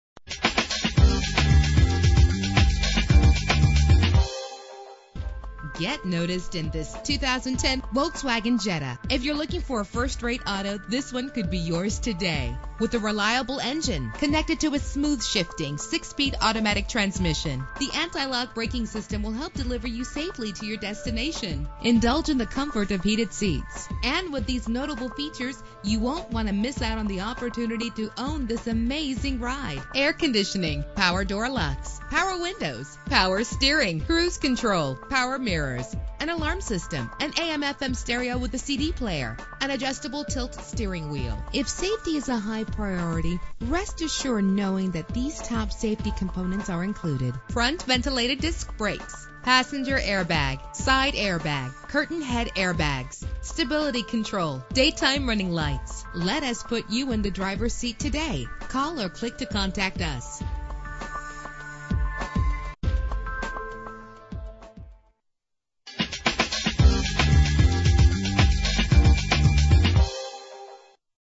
2010 Volkswagen Jetta - Feasterville PA
Year: 2010
Make: Volkswagen
Model: Jetta
Trim: S
Engine: 2.5 liter inline 5 cylinder 20 valve
Transmission: 6-Speed Automatic
Color: Platinum Gray
Mileage: 10
Address:
200 West Str